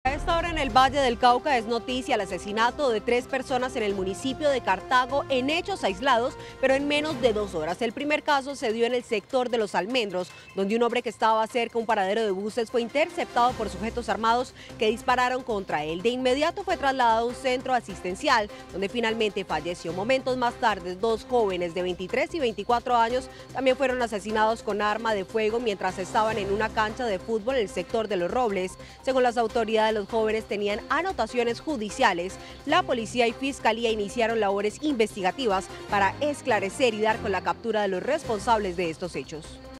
En un solo día matan a tres hombres en el municipio de Cartago, Valle
Los crímenes se cometieron en menos de dos horas. Uno murió cerca de un paradero de buses y a los otros dos los mataron en una cancha. Según las autoridades de Cartago, estos últimos tenían anotaciones judiciales.